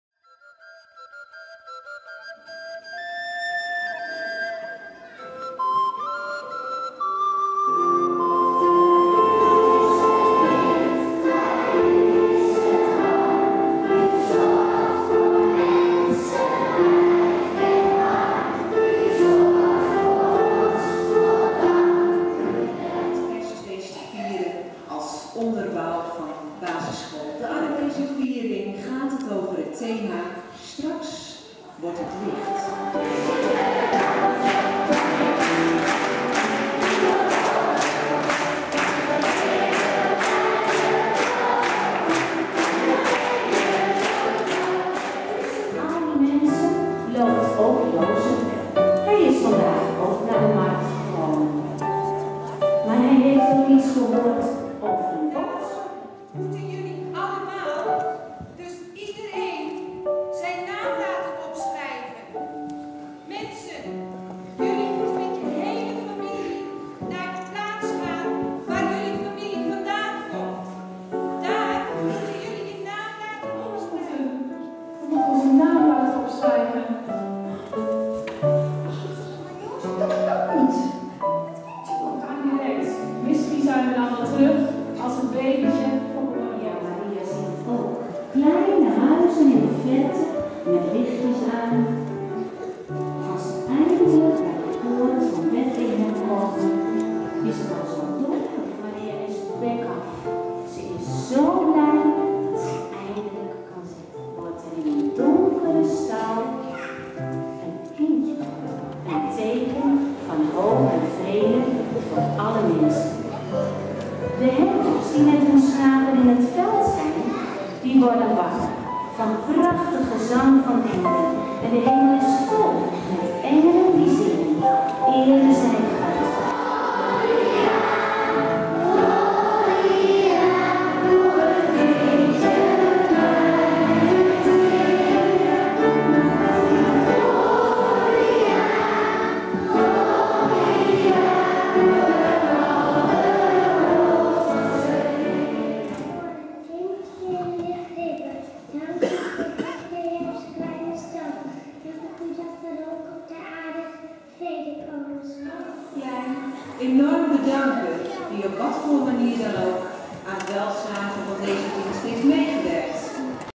Kerstfeest CBS De Ark groep 1 - 4
voor meer film en foto's
Donderdag 20 december 2012
Groep 1 t/m 4 van CBS De Ark vierde kerstfeest in de van Dedemkerk dat als thema deze avond had: straks wordt het licht. Elke groep had een bijdrage in deze kerstviering. Er werd volop gezongen en het kerst verhaal werd verteld en uitgebeeld door de leraressen van de groepen.